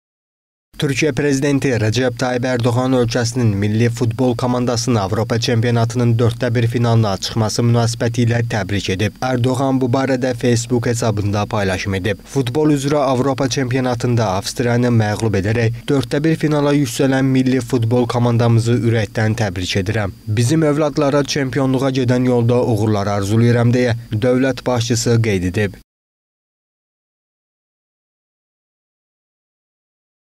"Çempionluğa gedən yolda uğurlar arzulayıram" – APA TV
Türkiyə Prezidenti Rəcəb Tayyib Ərdoğan ölkəsinin milli futbol komandasını Avropa çempionatının 1/4 finalına çıxması münasibətilə təbrik edib.

APA-nın məlumatına görə, Ərdoğan bu barədə "Facebook" hesabında paylaşım edib:

"Futbol üzrə Avropa Çempionatında Avstriyanı məğlub edərək, 1/4 finala yüksələn Milli Futbol Komandamızı ürəkdən təbrik edirəm. BizimÇocuklar’a çempionluğa gedən yolda uğurlar arzulayıram" - dövlət başçısı qeyd edib.